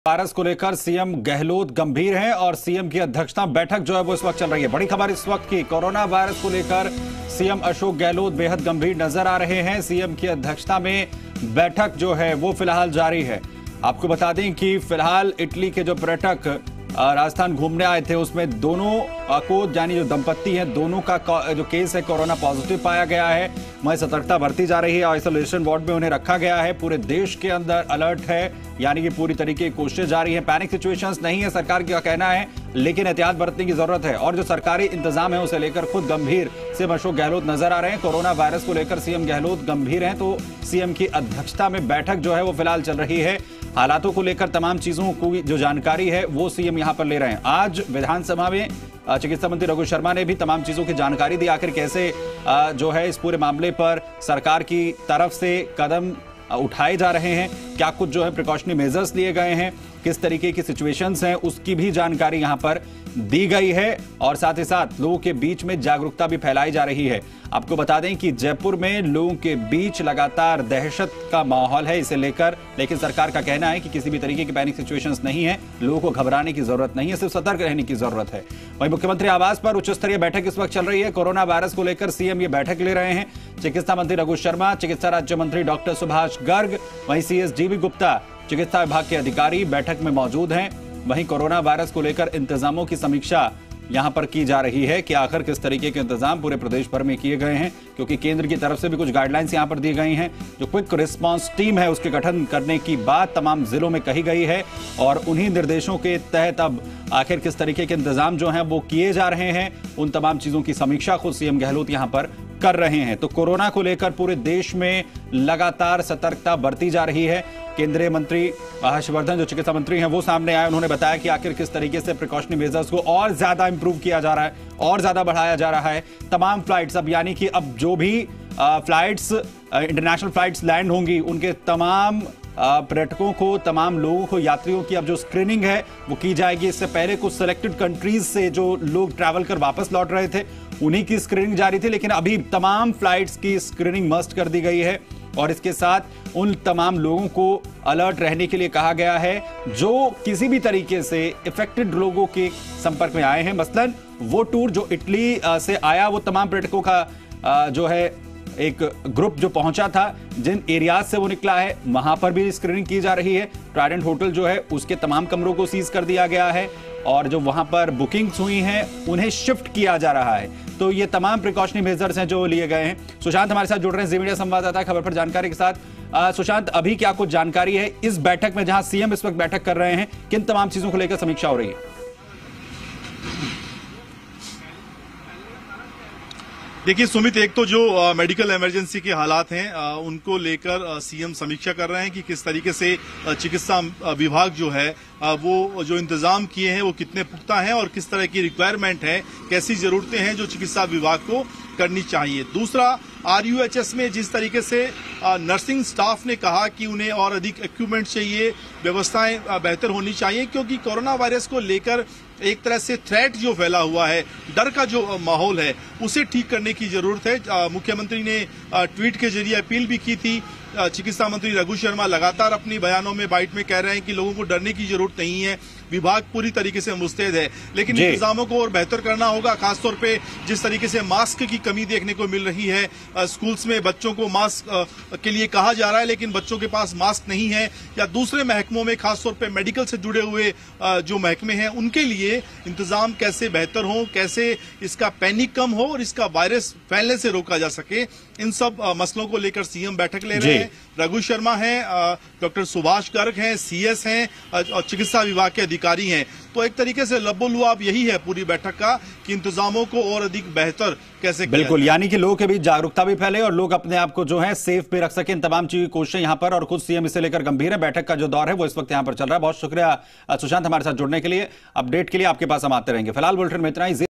Corona virus | Jaipur | कोरोना वायरस को लेकर सीएम गहलोत गंभीर
जयपुर- मुख्यमंत्री आवास पर उच्चस्तरीय बैठक। कोरोना वायरस (Corona virus) को लेकर सीएम ले रहे हैं बैठक। चिकित्सा मंत्री रघु शर्मा ( Raghu Sharma ) , चिकित्सा राज्य मंत्री डॉ सुभाष गर्ग ( Dr. Subhash Garg ) , सीएस डीबी गुप्ता (DB Gupta) चिकित्सा विभाग के अधिकारी हैं बैठक में मौजूद। मुख्यमंत्री कर रहे हैं कोरोना वायरस को लेकर इंतजामों को लेकर समीक्षा।